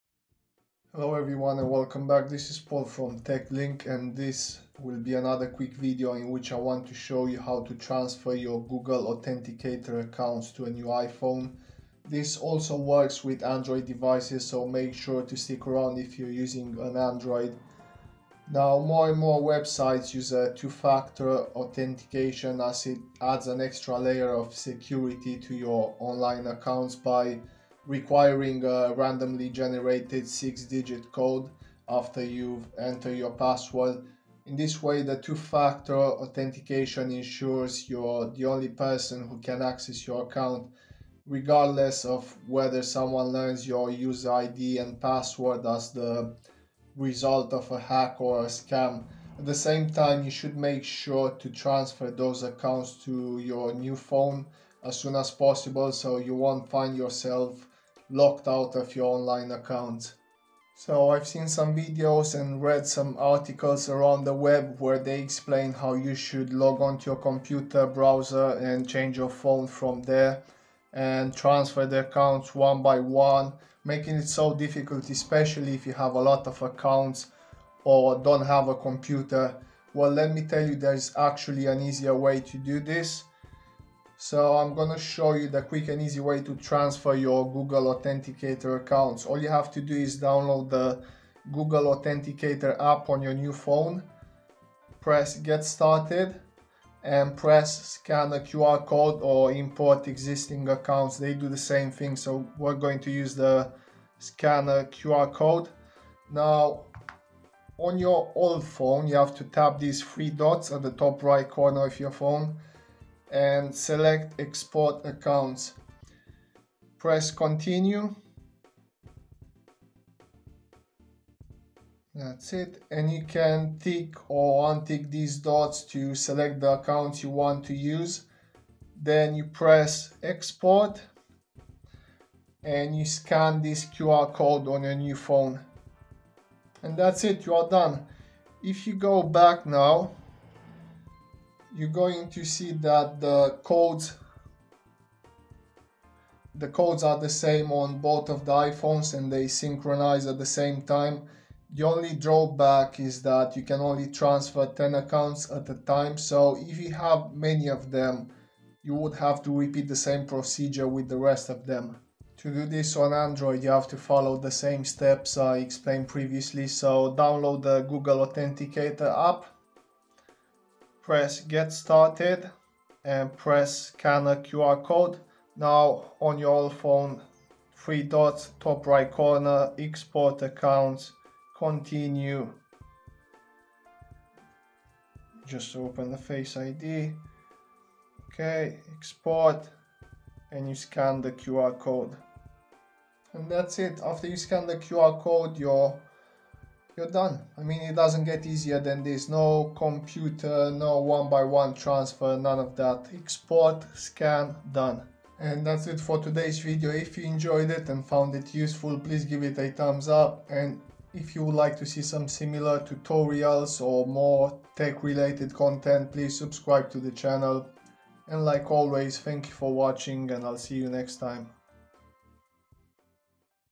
How to Transfer Google Authenticator Accounts to New iPhone or Android | Quick and Easy (2023)
In this video I want to show you how to transfer your google authenticator accounts from your old iPhone to a new one the quick and easy way. This also works with android devices so make sure to stick around if you're using an android phone.

More and more websites use a two factor authentication (2 FA) so you should make sure to transfer those 2 FA accounts to your new phone as soon as possible so you won't find yourself locked out of your online accounts.

0:00 Intro
1:24 How to Transfer Google Authenticator Accounts to New iPhone
2:42 How to Transfer Google Authenticator Accounts to New Android Phone
3:25 Outro

Support TechLink on Amazon